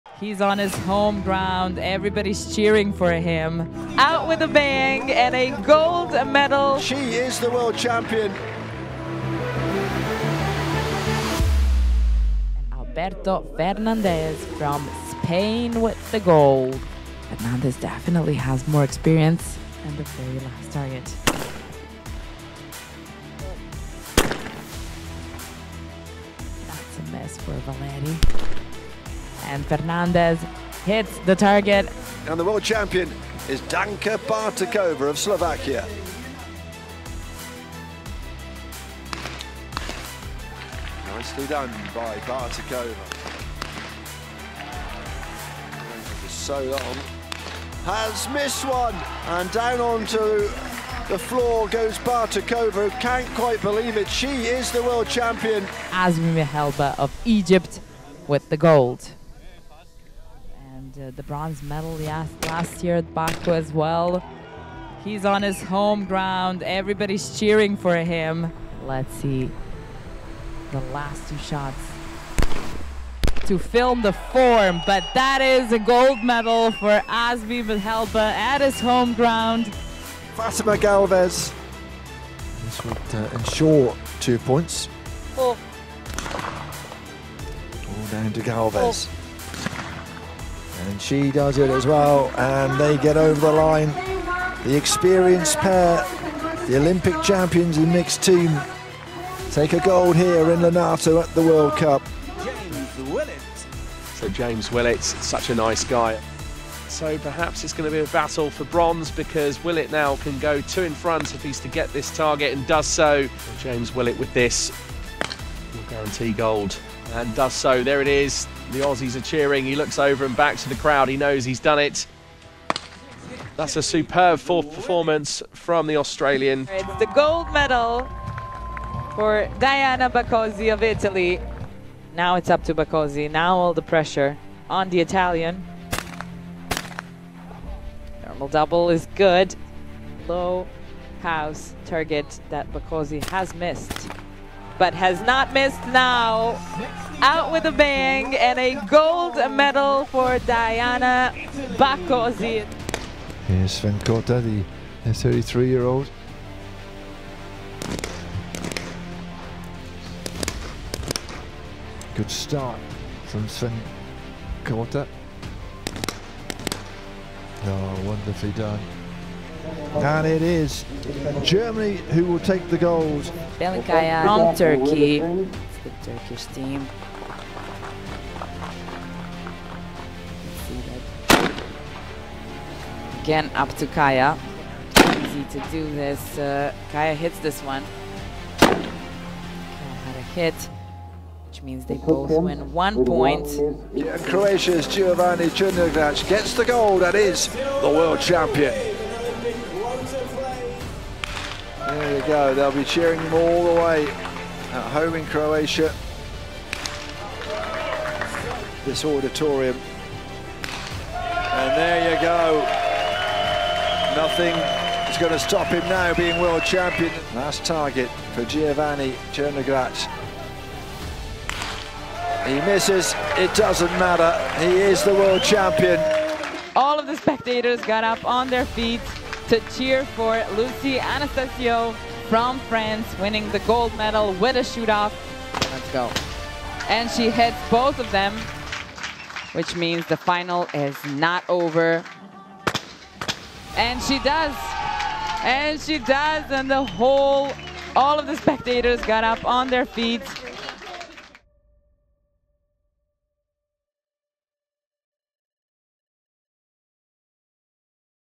Paris 2024: Shotgun Athletes To Watch | HitTheMark | ISSF
Shotgun events at Paris 2024 are set to be a spectacle, with the world's very best athletes set to battle it out for Olympic gold!

Men's Trap will see #1 ranked Alberto Fernandez look to add an individual gold to his tally, with #2 ranked James Willett looking to upset him.

In Women's Skeet 2x Olympic Medalist Diana Bacosi will look to reclaim Olympic gold but will have to beat out the likes of #3 ranked Lucie Anastassiou and Danka Bertekova!